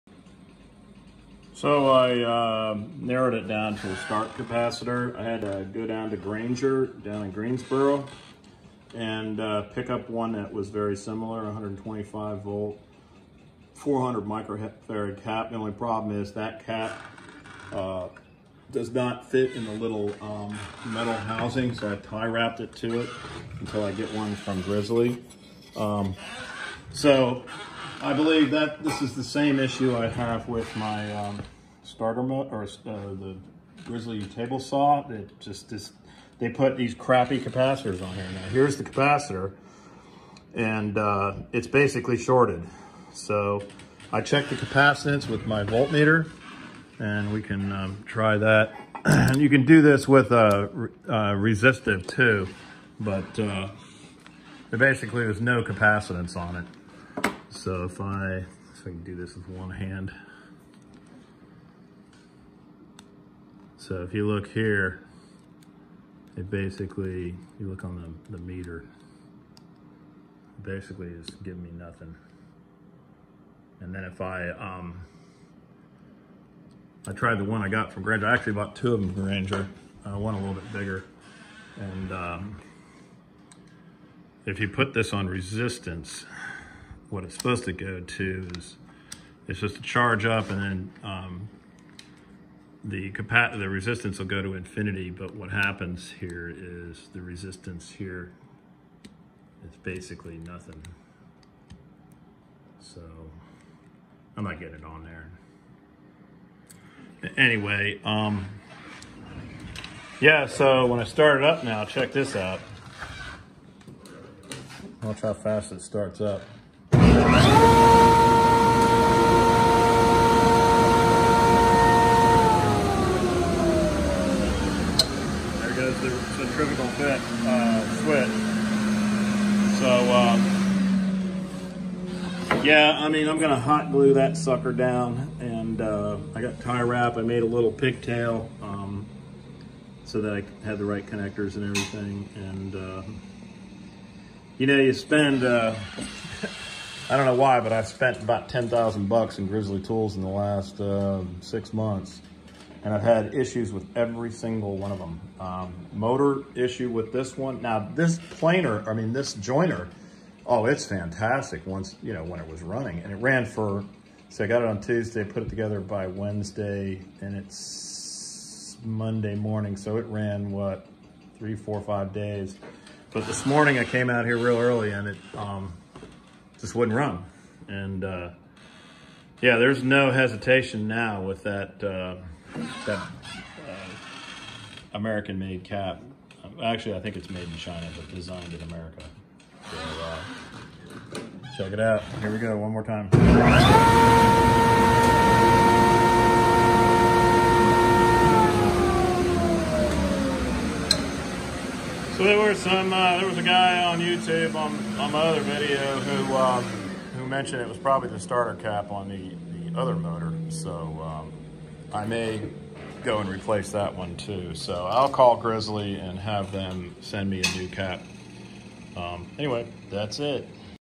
New Grizzly G0495X jointer starter cap failure and temp replacement.
Brand new grizzly G0495X jointer started having startup problems after about four days of operation. The motor would turn on very very slowly and it would then would quickly shut down.  After looking at the Centrifugal switch  to make sure that was set up  properly,  I went to the starter cap and measured it.  I first used a volt meter on the resistance setting and it went to zero after a second or two.  It should go high Z to or essentially infinity. Then I used my fluke volt meter using the capacitance setting.  On the capacitance setting it read 0 UF.  I’m guessing it was either short or not connected.  I’m thinking it was the former (shorted).  So I went down to the local Granger supply and picked up a similar value cap of voltage and capacitance (125V/400UF) and temporarily zip tied and hot glued it to the motor.  I made a pigtail so the contacts were good and put heat shrink over the leads so no shorting could occur.  One issue was The cap I bought was mechanically a bit larger than the cheap ass skinny ones Grizzly adds on these motors. The new cap  would not fit under the cap cover and hence the zip tie and hot glue mounting.  I’ll call Grizzly and have them send me a new cap later today.   In the mean time, the saw now spins up quickly and doesn’t shut down.  As far as how the jointer cuts, I couldn’t be happier.   This starting issue is a similar issue I have on my new Grizzly G0605X 12 inch table saw.  To me, Skimping on starter caps IMO is effing BS when the machine costs north of $2700, I would expect better.  I will
Most likely never buy a Grizzly again. (Famous last words ). - Porch